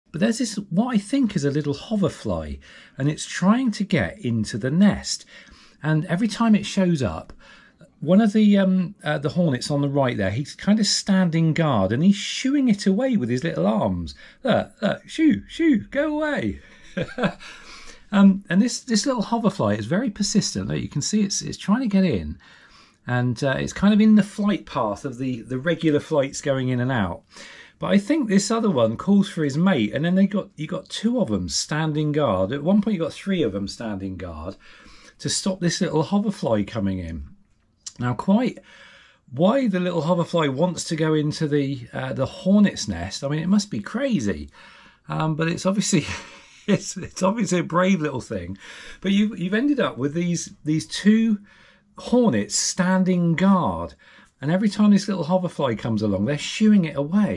Hornets Nest Vs Hoverfly: Surprising Behaviour
A hoverfly tries to enter a hornet's nest...